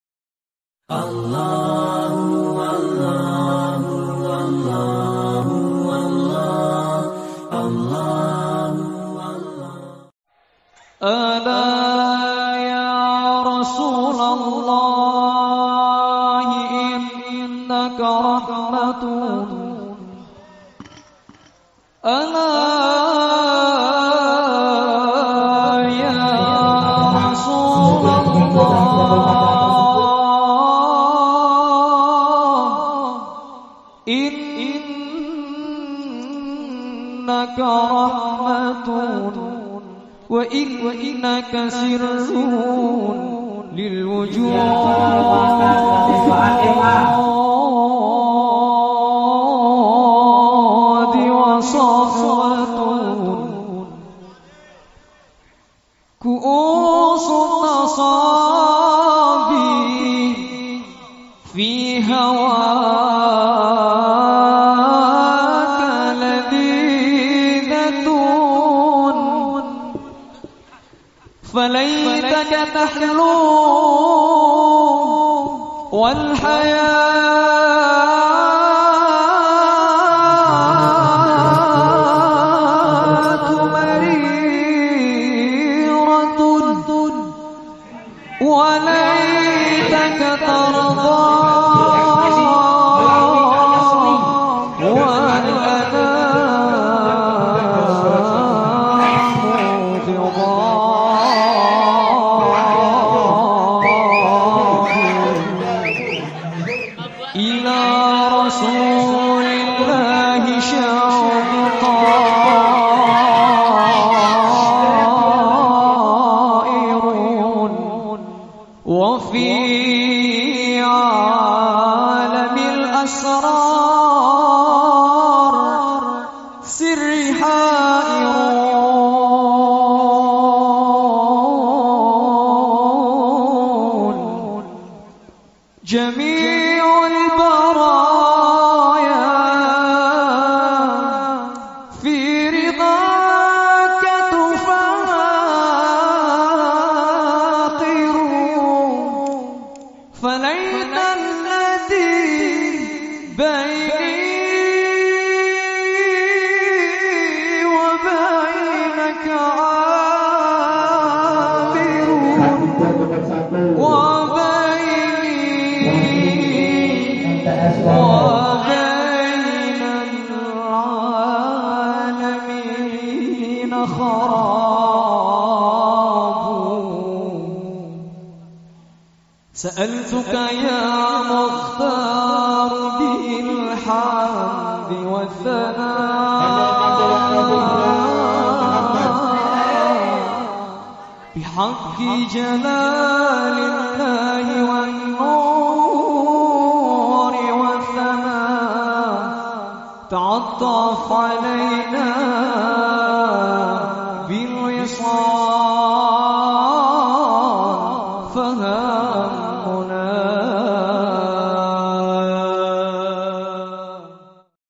MPLS Daruttauhid Jepara
Waktu dimana santri baru diperkenalkan dengan lingkungan sekolah dan pondok supaya mereka bisa beradaptasi dan bisa lebih betah dip pondok daruttauhid jepara